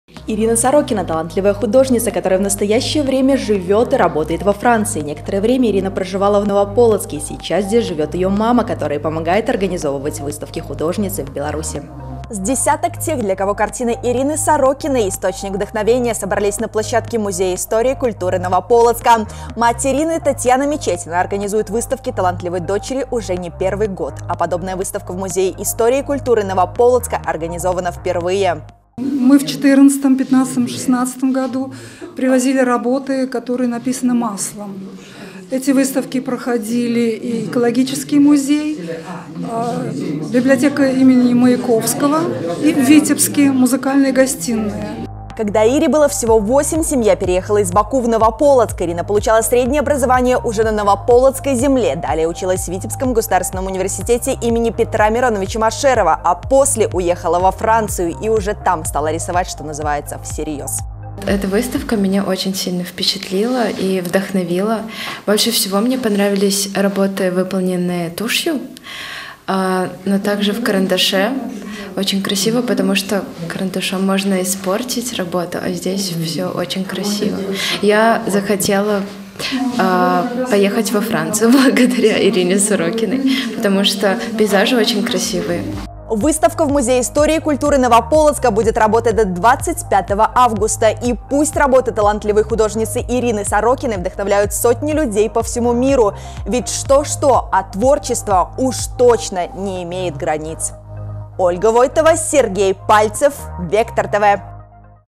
Выставка Ирины Сорокиной
Семья Ирины Сорокиной, талантливой художницы, переехала в Новополоцк тогда, когда девочке было 8 лет. Сейчас работы Ирины можно наблюдать в коллекциях Франции,  Германии, Индонезии и Соединённых Штатов Америки. Персональная выставка впервые приехала в Новополоцке. С подробностями наша съёмочная группа.